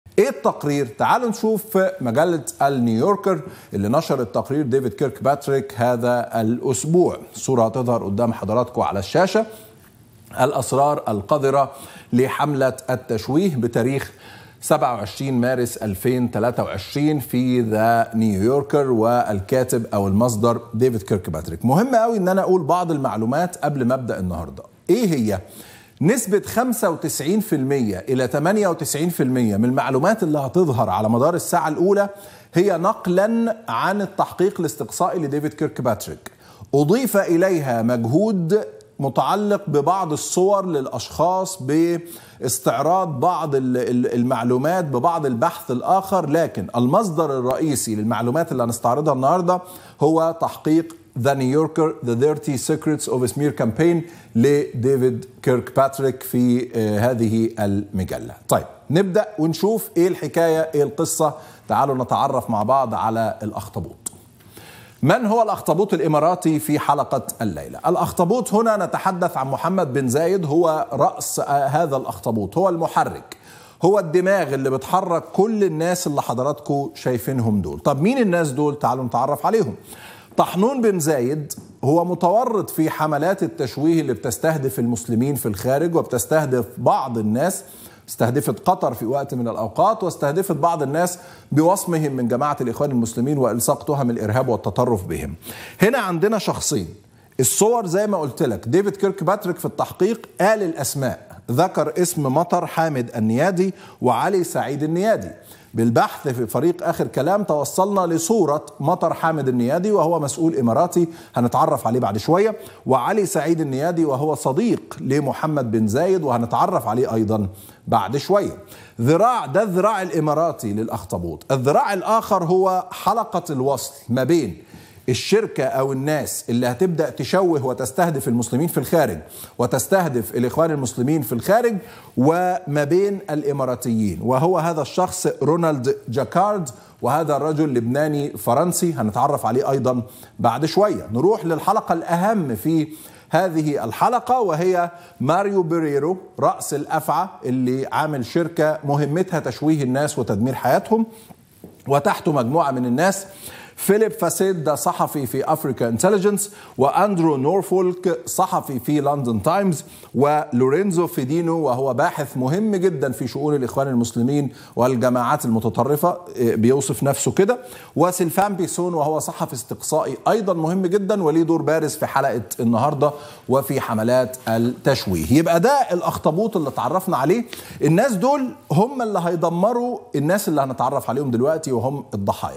أسامة جاويش يفضـ ـح الأذرع الرئيسية للأخطبوط "محمد بن زايد" على الهواء!
يكشف أسامة جاويش  الأذرع الرئيسية للأخطبوط "محمد بن زايد" والأسرار القـ ـذرة لحملات الإمارات السوداء على المسلمين في أوروبا والتي يديرها محمد بن زايد و يكشف أبرز المستهدفين من قبل الأخطبوط الإماراتي